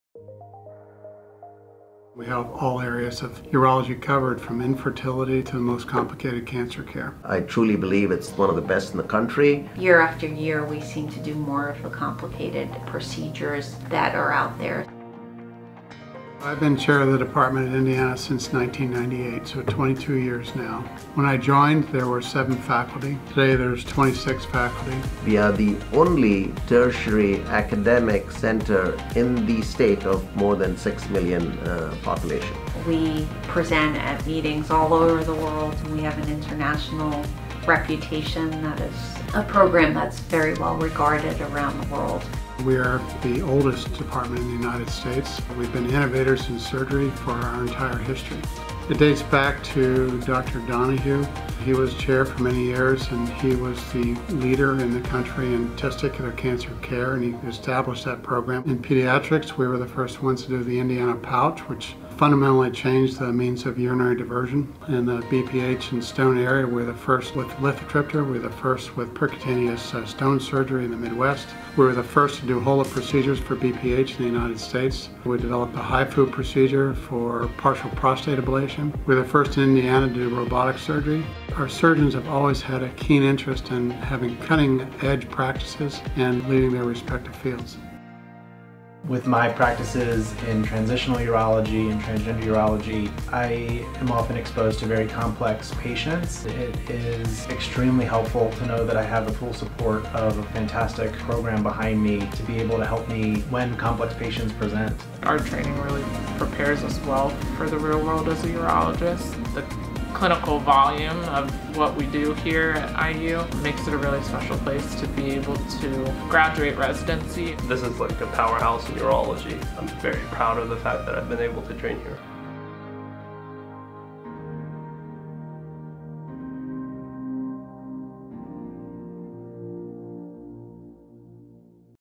IU School of Medicine Department of Urology
Learn about what makes IU School of Medicine Department of Urology one of the strongest and most innovative urology departments in the country.